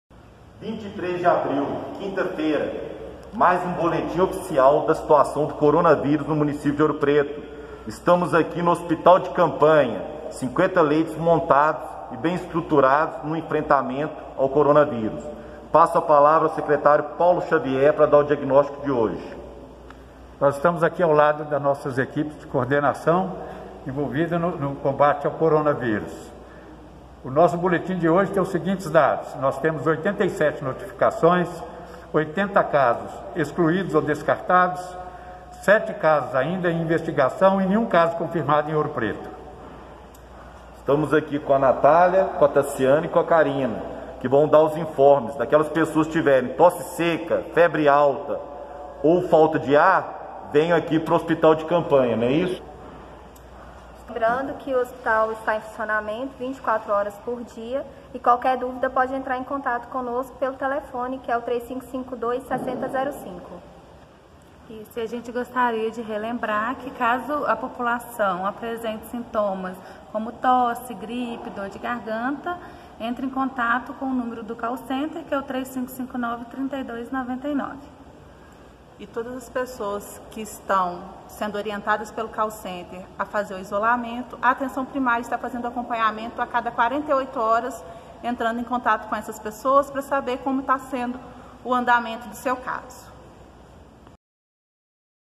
23/04/2020 – Edição 36 – Boletim sobre coronavírus em Ouro Preto
🎥 Boletim oficial da Prefeitura de Ouro Preto do dia 23 de abril (quinta-feira). ⚠️ Fique em casa! 😷